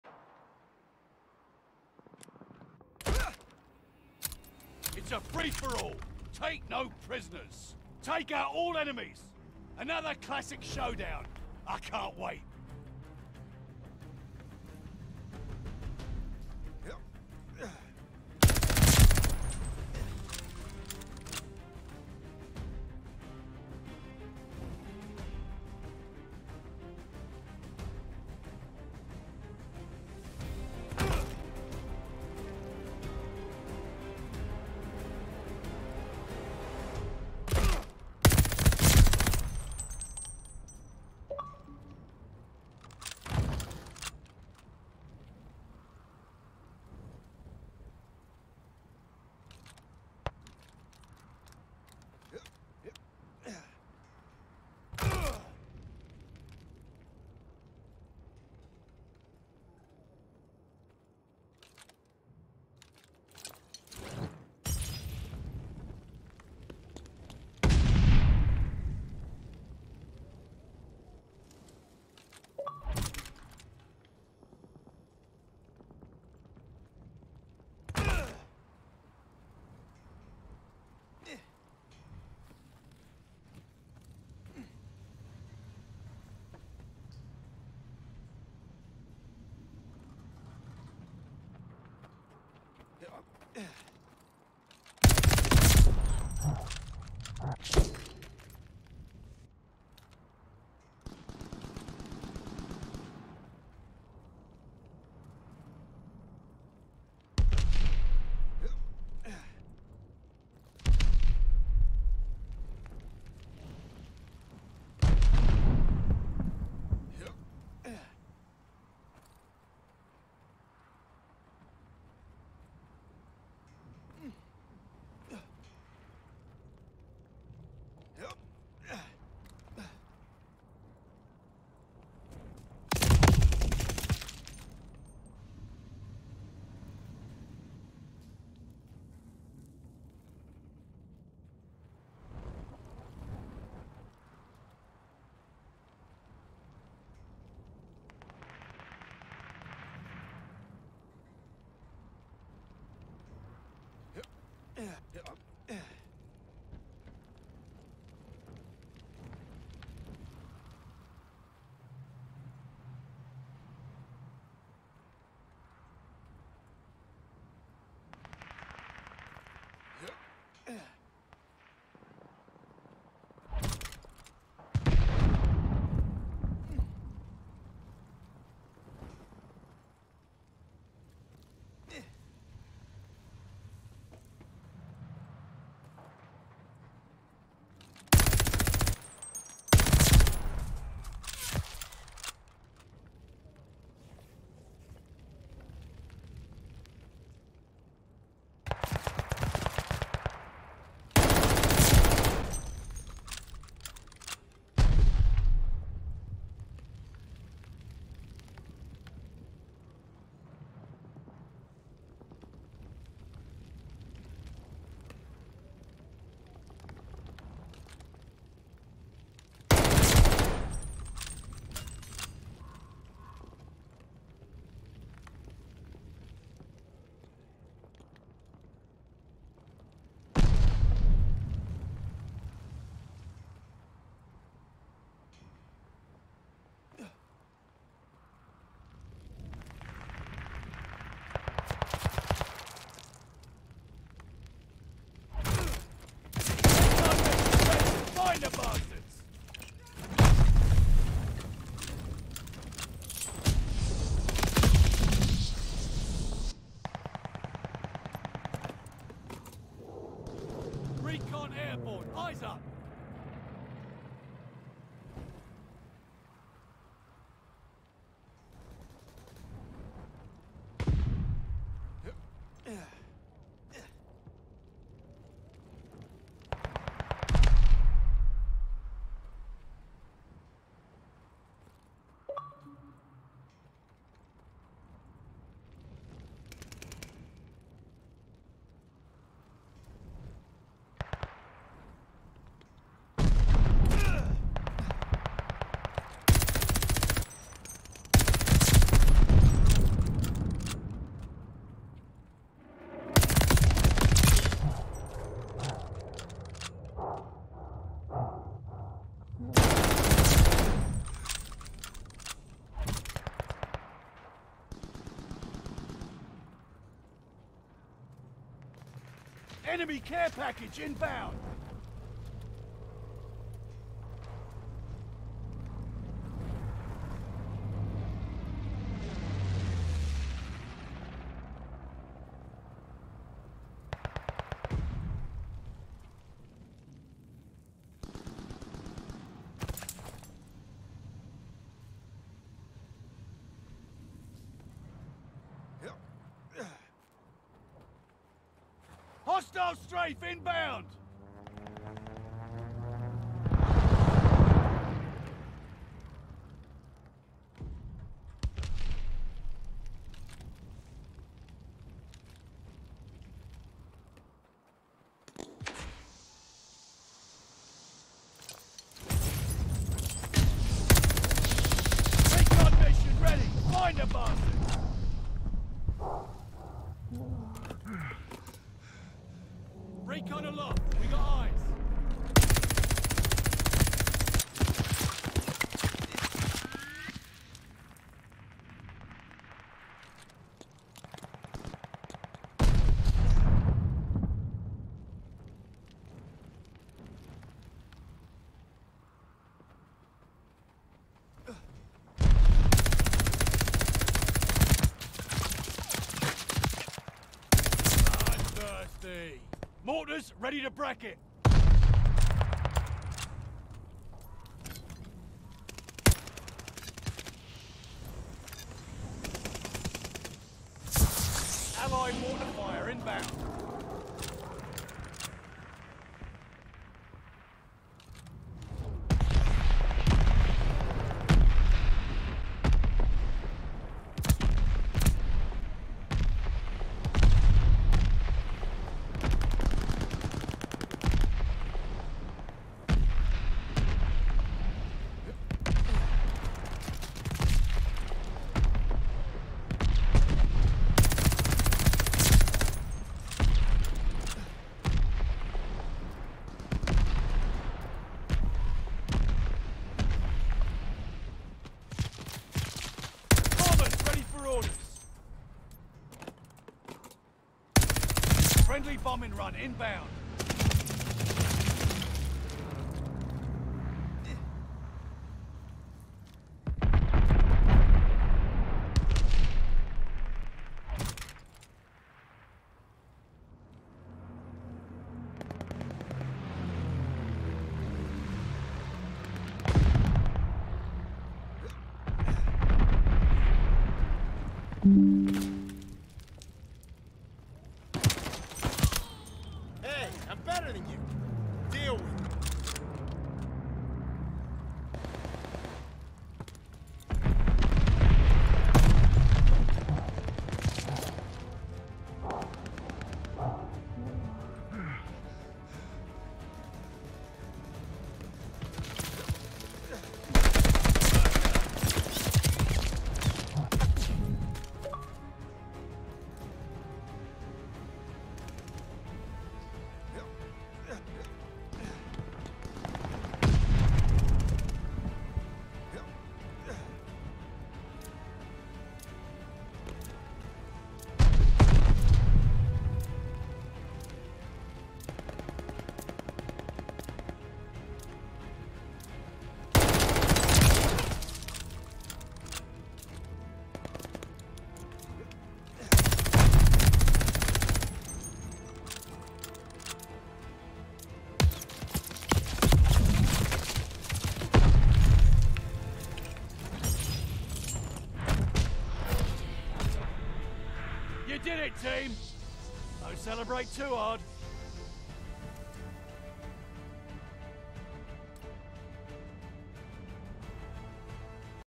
Map Red Star “nice win” on a great winter map
Map Red Star good win on this one!

 Weapons: AS44 and Knife
Combat Pacing: Assault

Found this one in my archives from November 18th, 2021
I forgot about this match but here it is early days of playing FreeForAll, and the AS44 was the shit at the time it's still a good rifle, but you know how that goes as more weapons either come out, or you upgrade others to use.

Enjoy the vid we all have come a long way since November gameplay lol